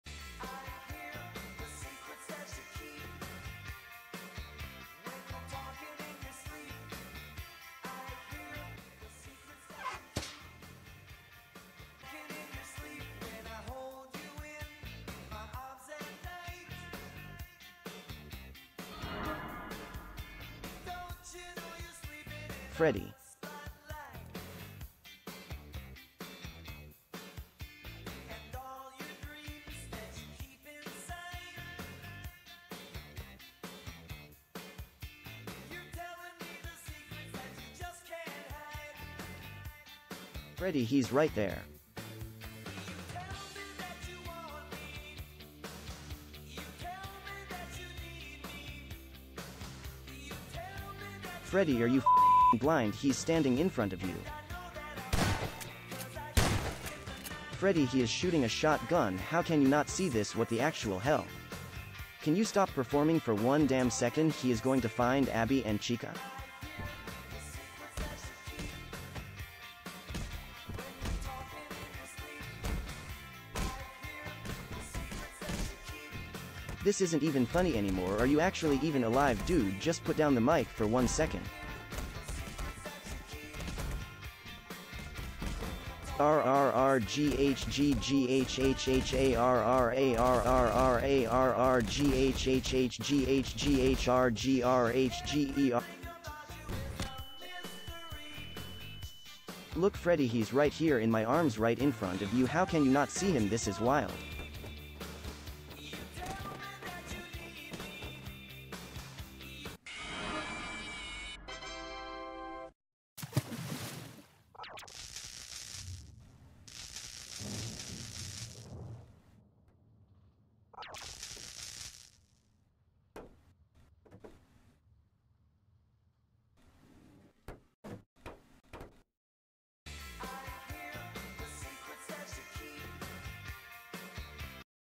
HE’S RIGHT THERE!!! - FNAF Bonnie Pointing Out Mike To Freddy Animation
The Five Night's at Freddy's Movie includes a funny moment in which Bonnie desperately tries to point out Mike sneaking in to Freddy Fazbear, however Freddy doesn't notice at all, and so todays animation was just a little funny joke about that moment. I made this myself so any critism is welcome and feedback too!